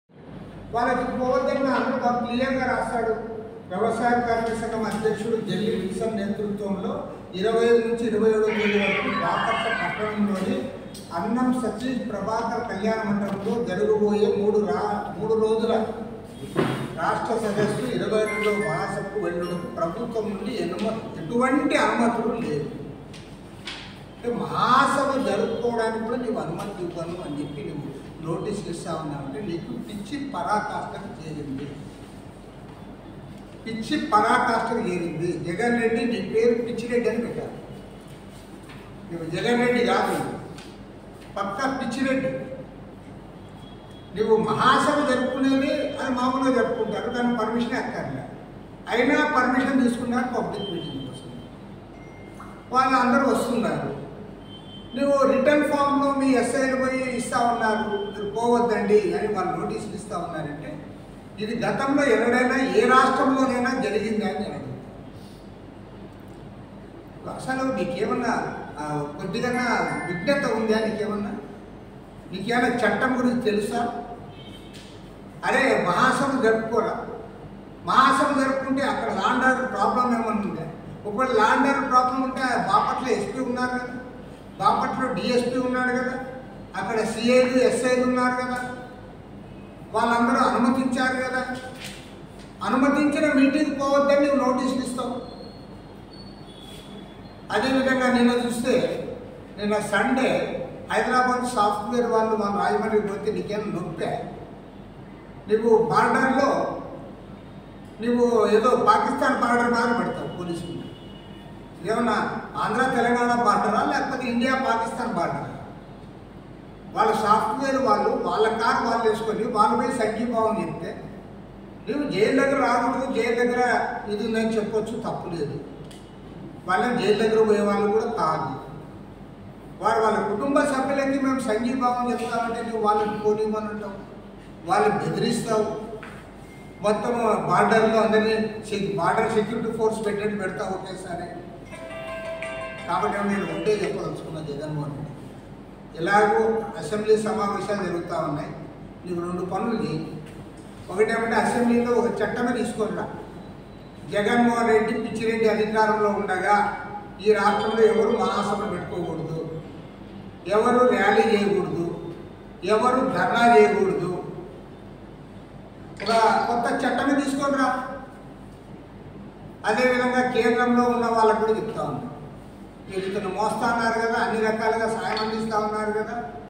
AP CPI Secretary K. Rama Krishna Sensational Comments On CM Jagan | Political Updates | Wild Wolf
Watch here